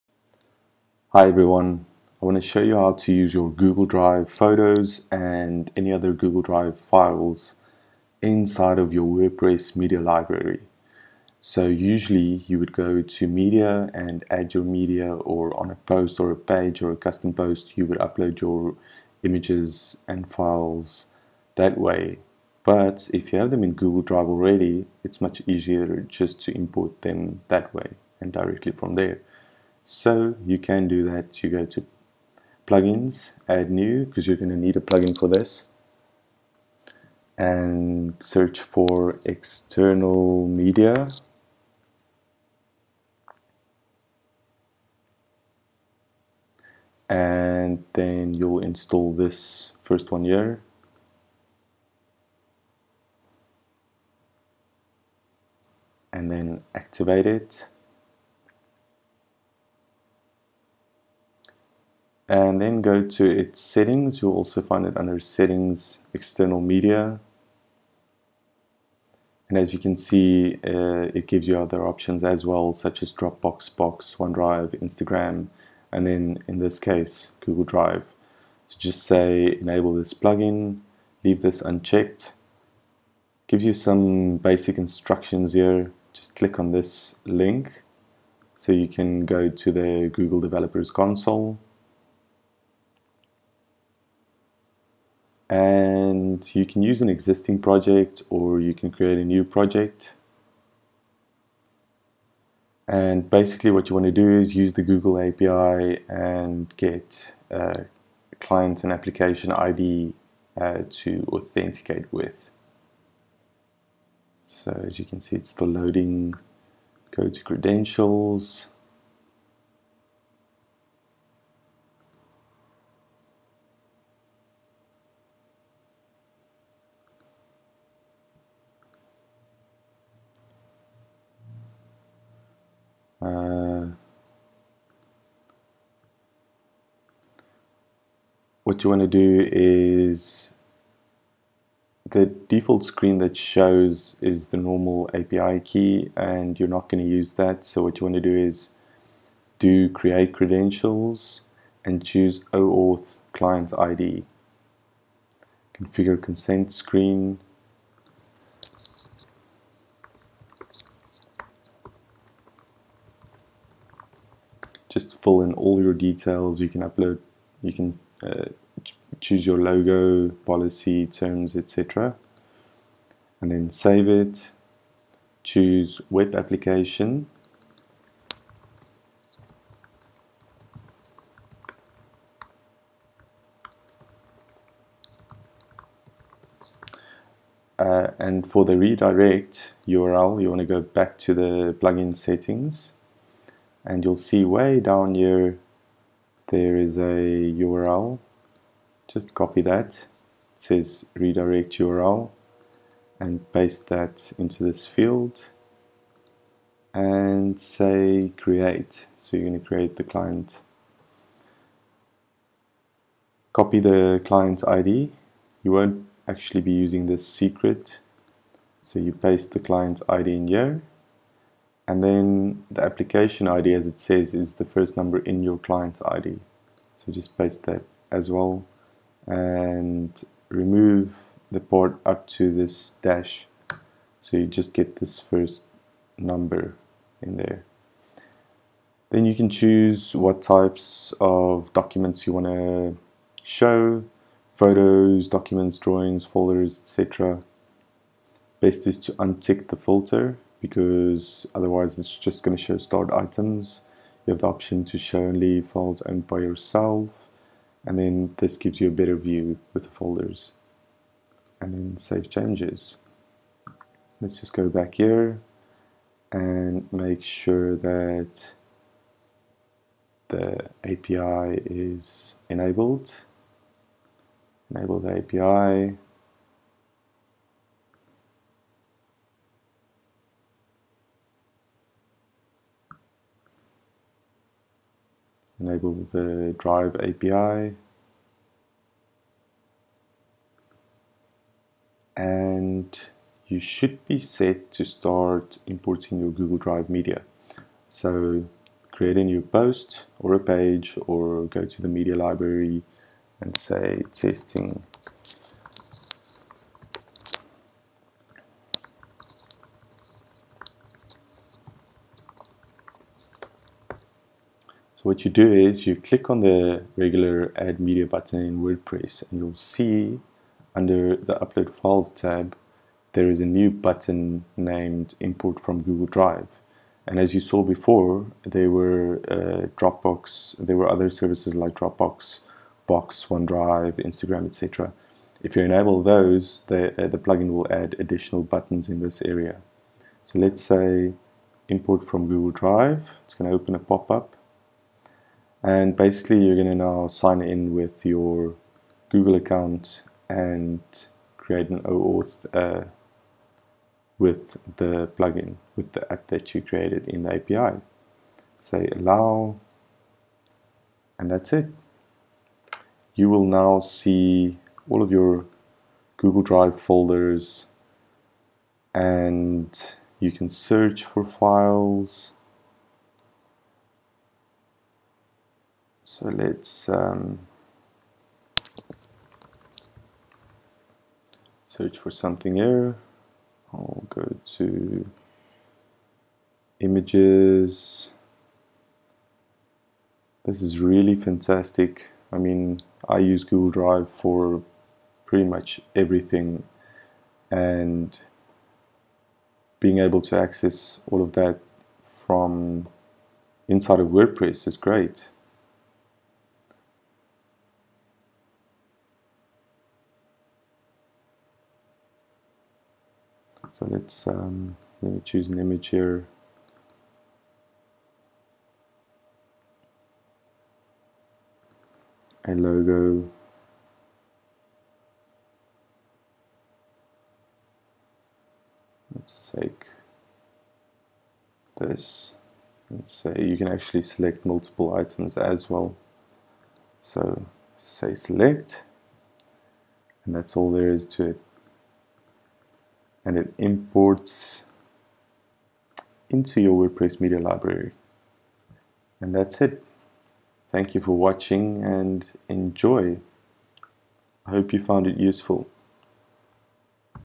Use Google Drive Photos and Files in WordPress Media Library
You can use your Google Drive photos and files in your WordPress media library. Import them from Google Drive into WordPress.